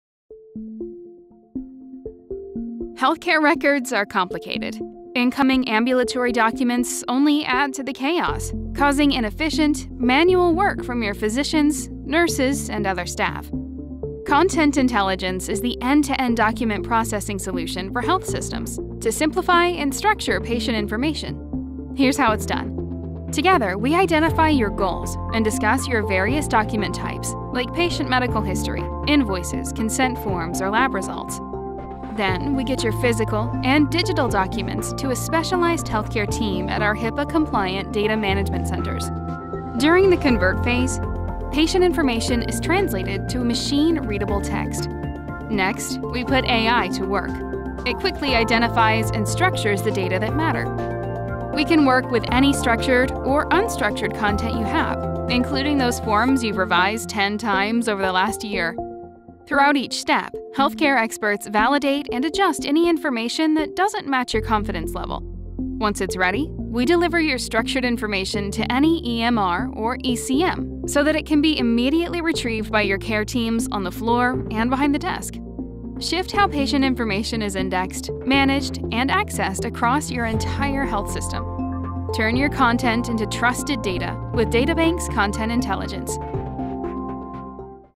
Using AI and machine learning to structure healthcare patient records
Intelligent Document Processing (IDP) can reduce many issues for healthcare operations departments, including inaccurate or unstructured medical records, manual indexing, lack of process, and more. See how DataBank leverages IDP and healthcare experts to create a 98% accurate medical record indexing system that follows HIPAA compliance, while advancing care through placing data in the right hands.